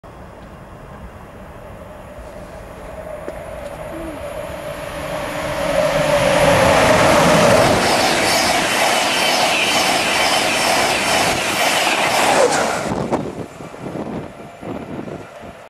National Express 225 flies through Prestonpans
Nearly takes me with it! Lot of turbulence off that one. Class 322 stopped at other platform.